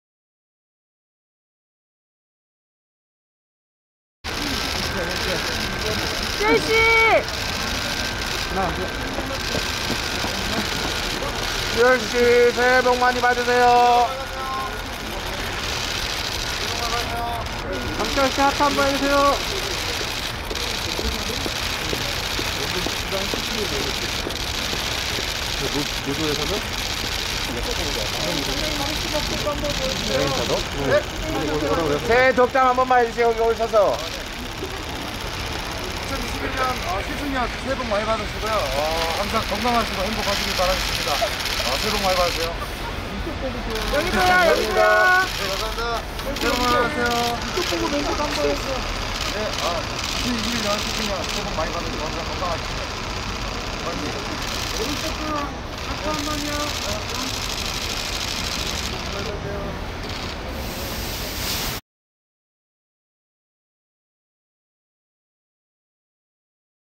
황치열 (Chiyeul Hwang), '즐거운 명절보내세요~' (트롯전국체전)
가수 황치열이 2월 10일 오전 KBS 2TV '트롯 전국체전' 녹화를 위해 서울 여의도 KBS 신관으로 들어서고 있다.